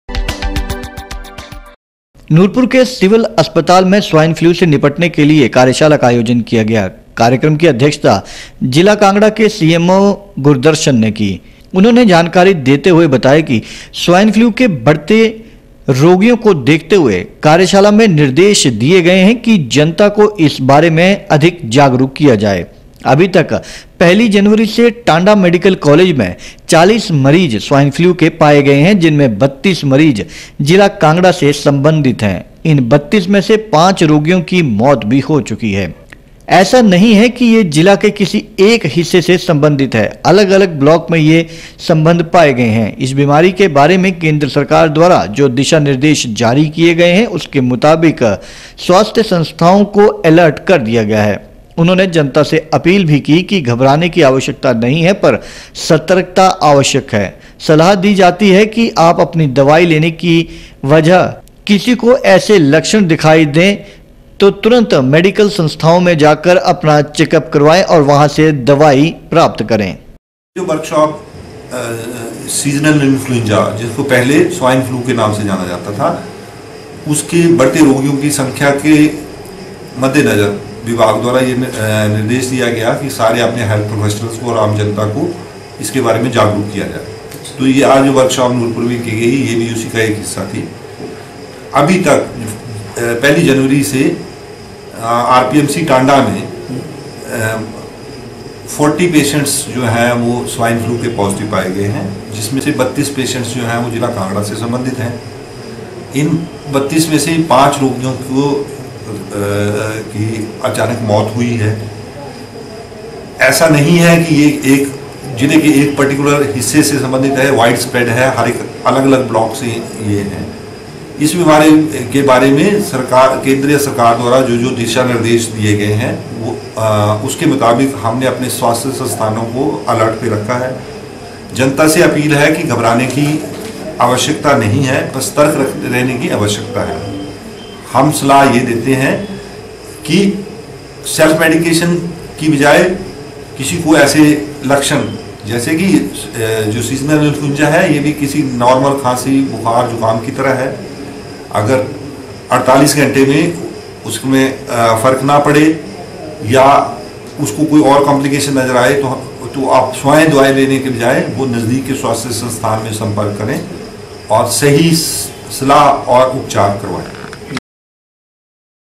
नूरपुर के सिविल अस्पताल में स्वाइन फ्लू से निपटने के लिए किया गया कार्यशाला का आयोजन
नूरपुर के सिविल अस्पताल में स्वाइन फ्लू से निपटने के लिए कार्यशाला का आयोजन किया गया।कार्यक्रम की अध्यक्षता ज़िला कांगड़ा के सीएमओ गुरदर्शन ने की।उन्होंने जानकारी देते हुए बताया कि जिसे पहले स्वाइन फ्लू के नाम से जाना जाता था के बढ़ते रोगियों को देखते हुए कार्यशाला के द्वारा निर्देश दिए गए कि जनता को इसके बारे जागरूक किया जाए